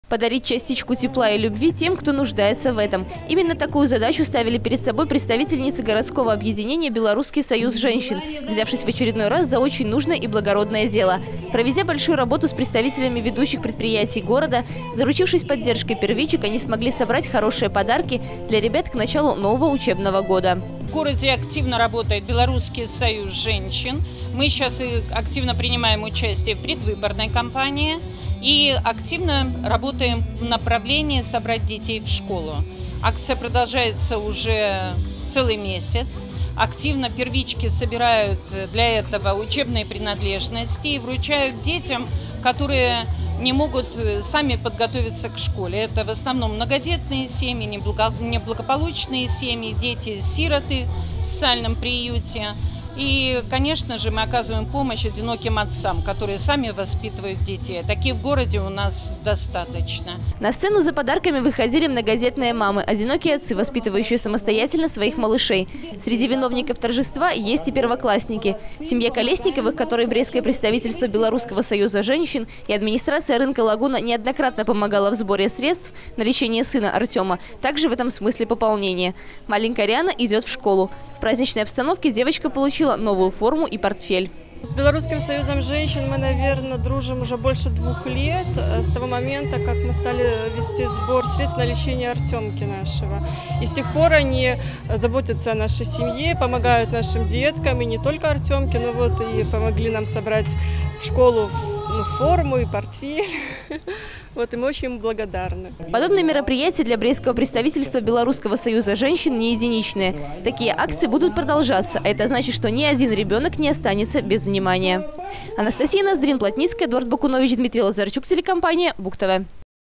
2016-08-30 г. Брест. Благотворительная акция на рынке «Лагуна». Новости на Буг-ТВ.
Благотворительная акция на рынке «Лагуна» от «Белорусского союза женщин».
Рынок «Лагуна» в микрорайоне «Ковалево» уже давно стал местом консолидации разного рода мероприятий. В очередной раз здесь прошло ещё одно благотворительное торжество. Небольшая акция переросла в настоящий праздник, где было место не только подаркам, но и сюрпризам от юных артистов областного центра. Заглянула «на огонек» и наша съёмочная группа.